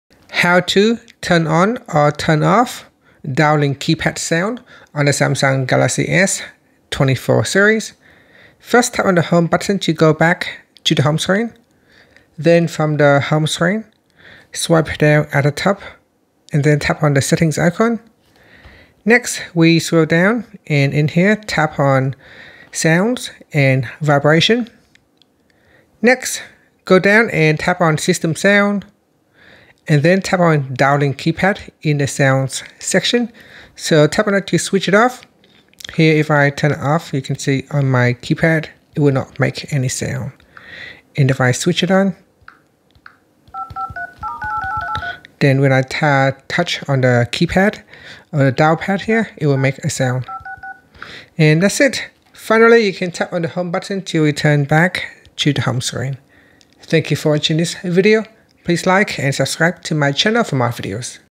Galaxy S24/S24+/Ultra: How to Turn On/Off Dialing Keypad Sound
Those dialing pad beeps driving you crazy? Or do you miss the confirmation of each tap? This video shows you how to easily turn on or off the dialing keypad sounds on your Galaxy S24, S24+, and Ultra running Android 14! Whether you prefer a silent dialing experience or the reassuring clicks, customize your keypad sounds in seconds with this simple setting. Discover the benefits of both options and find your perfect dialing vibe!

What you'll learn:

- How to quickly find the setting to control dialing sounds.
- Why you might want the sounds on or off (it's up to you!).